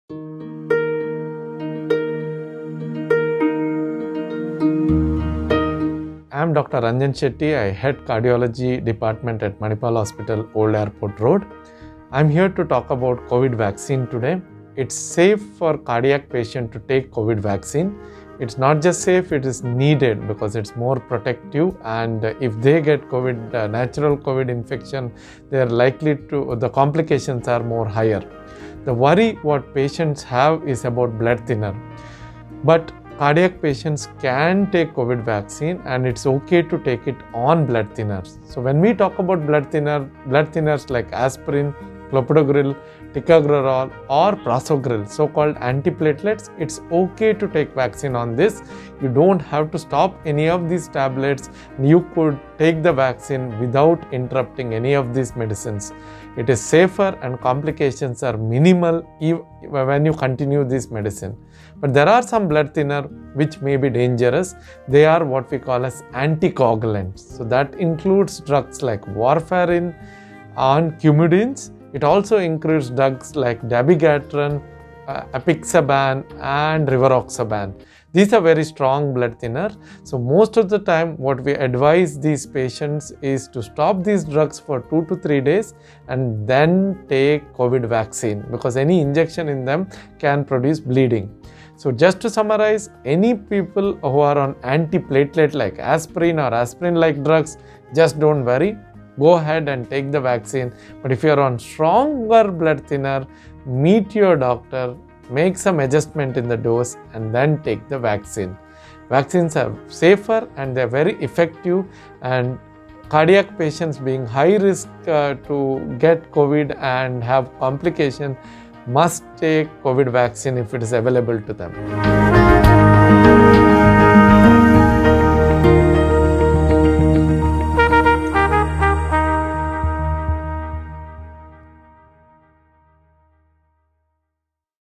Dr. Ranjan Shetty on COVID-19 vaccine for Heart Patients | Manipal Hospitals India
Dr. Ranjan Shetty, HOD and Consultant - Interventional Cardiology, Manipal Hospitals, Old Airport Road, explains that the COVID-19 vaccine is safe for heart patients. He also talks about how the patient must consult the doctor before getting vaccinated if they are on stronger blood thinners. Watch the video to know more! To know more about Dr. Ranjan Shetty